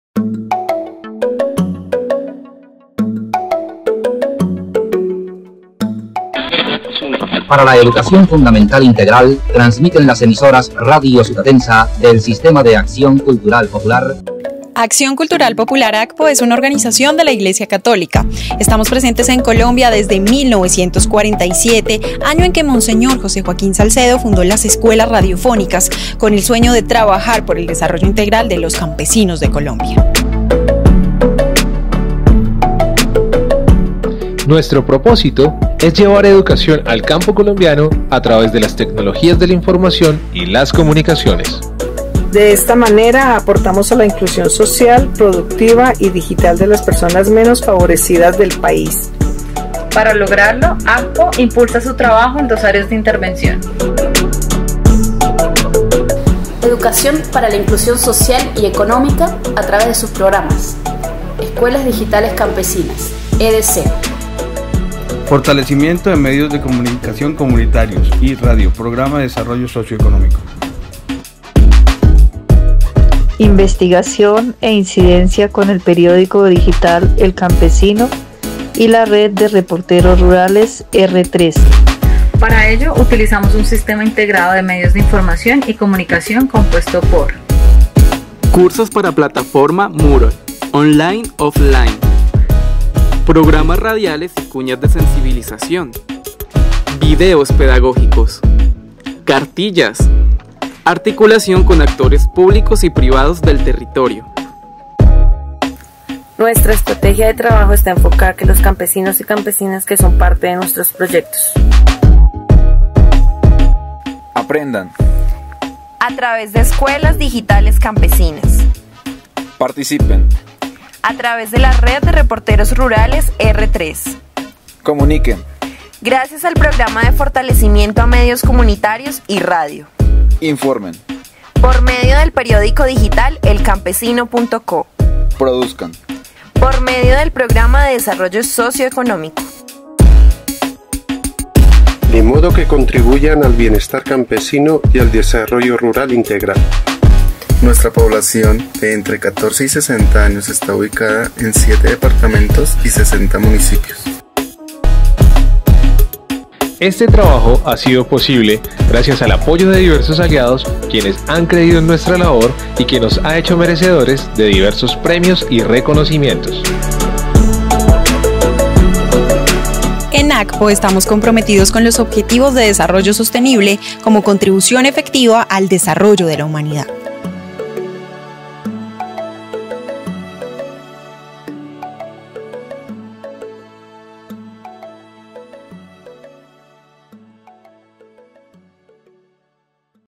Somos Acción Cultural Popular | Sembramos Educación
En ACPO trabajamos en programas y proyectos que promueven el desarrollo y el bienestar de campesinos y campesinas a través de la educación para su plena inclusión social y económica. Conoce en este vídeo toda nuestra historia y anímate a ser parte del cambio